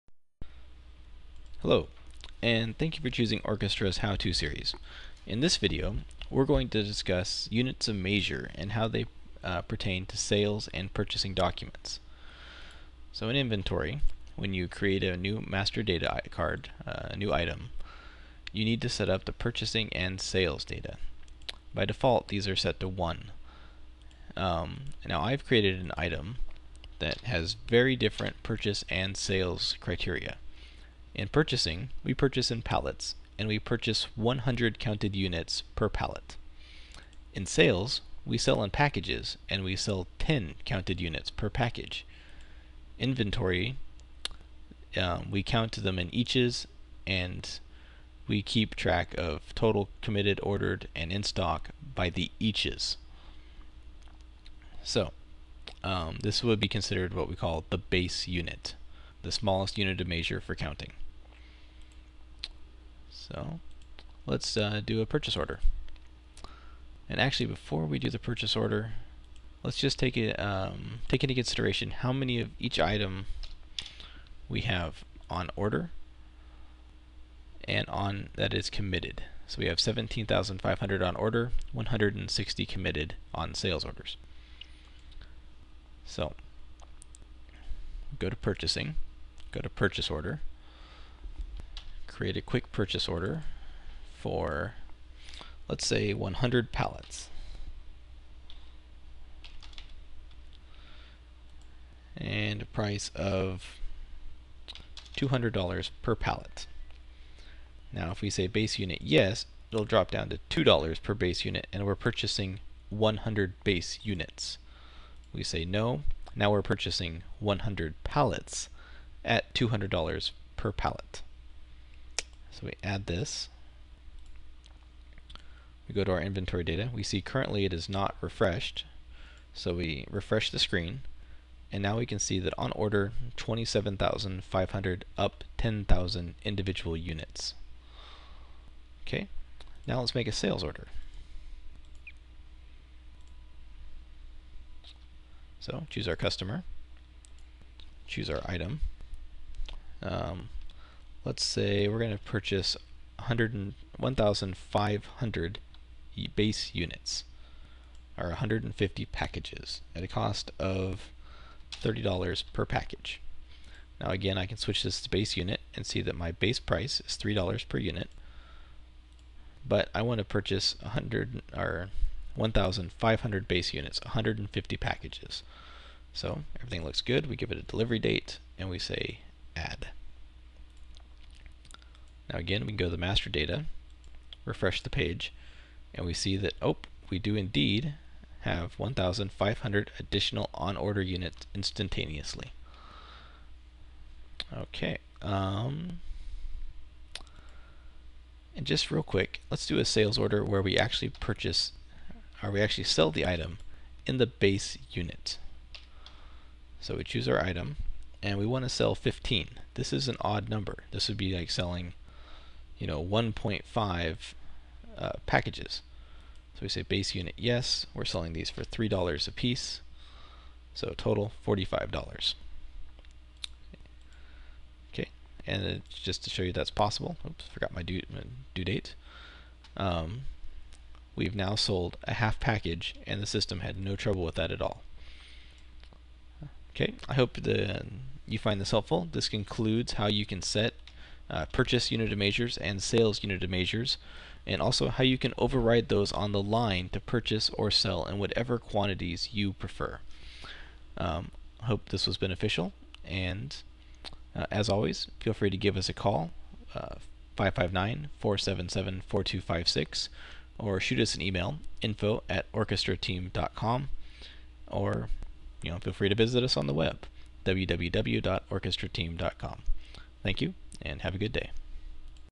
SAP Business One How To Manage Purchasing and Sales Units of Measure
In this video we discuss how to manage your purchasing and Sales units of measure as well as how to over ride them on the line for special orders or partial package sales.